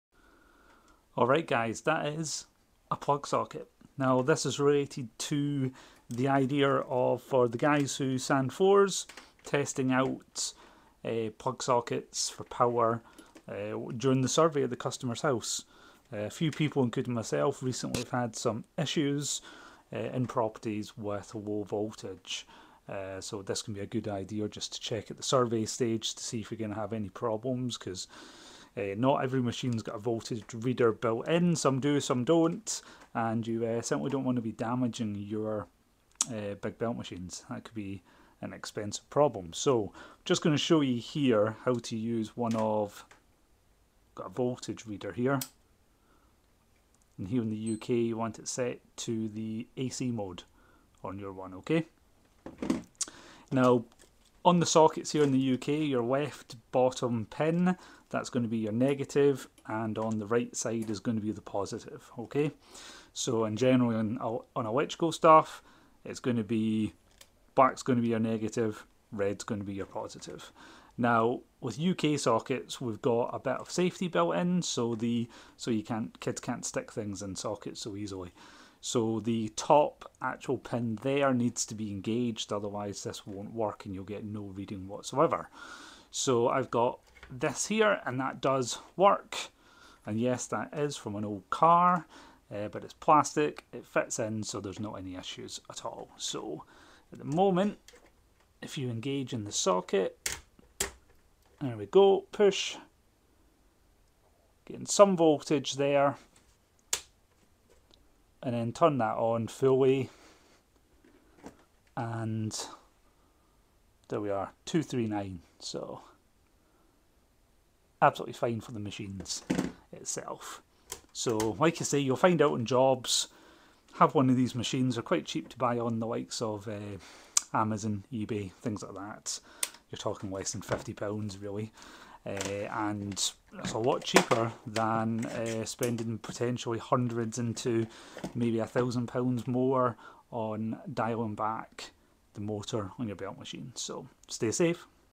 Testing Voltage on Your UK Power Socket
Testing voltage on the sockets you use Belt sanding machines and other high powered machines is very important.  If the voltage is too low or other problems exist then you can damage parts of the machine which can end up an expensive repair.
This is an easy way to to test.  Most voltage readers can be purchased for less than £50.